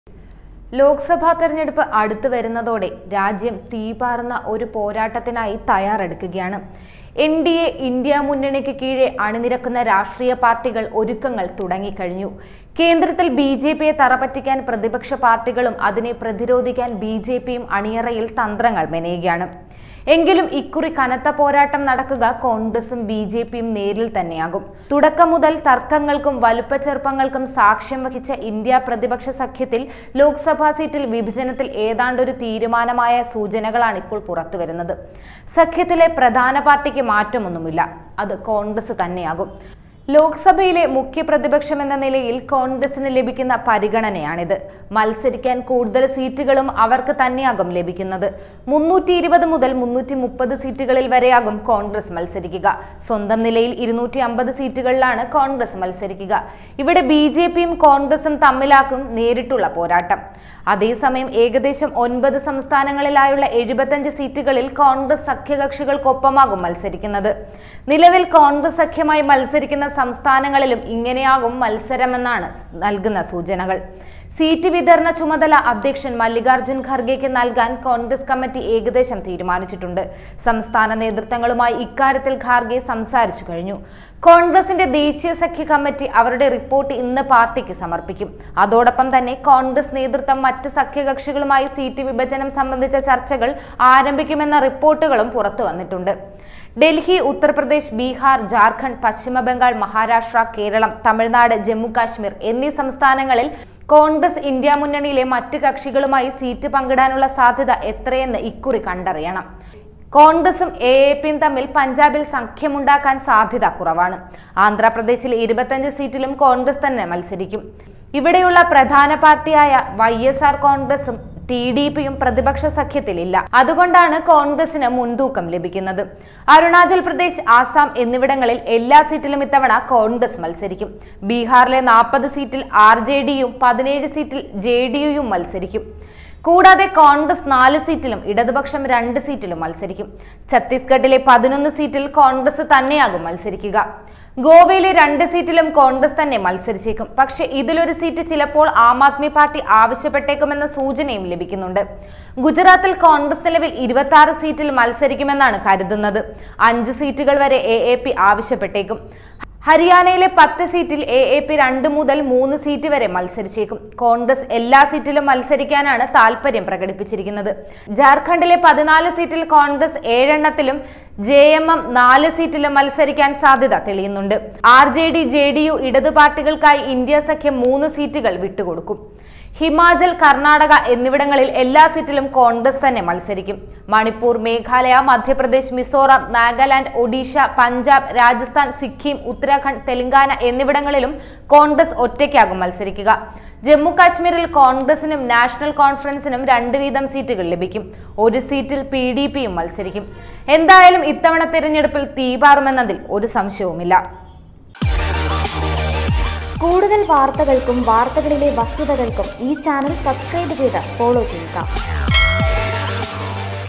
രണ്ടു സീറ്റിൽ ഇടതുപക്ഷം വിജയം ഉറപ്പിച്ചുകഴിഞ്ഞു; ഇന്ത്യ സഖ്യത്തില്‍ സീറ്റ് വിഭജനം ഇങ്ങനെ
ഇന്ത്യ സഖ്യത്തില്‍ ലോക്‌സഭ സീറ്റ് വിഭജനത്തില്‍ തീരുമാനമായെന്ന് സൂചന. സഖ്യത്തിലെ പാര്‍ട്ടി കോണ്‍ഗ്രസ് തന്നെയായിരിക്കും. . 320 മുതല്‍ 330 സീറ്റുകളില്‍ വരെയാകും  കോണ്‍ഗ്രസ് മത്സരിക്കുക . ലോക്‌സഭയിലെ മുഖ്യ പ്രതിപക്ഷമെന്ന നിലയില്‍ കോണ്‍ഗ്രസിന് ലഭിക്കുന്ന പരിഗണനയാണിത്.